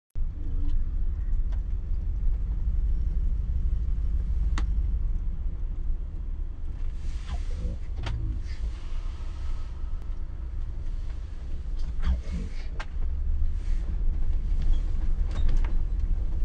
Алматы. ДТП на аль - Фараби - Жарокова.
В городе шел 350 день карантина.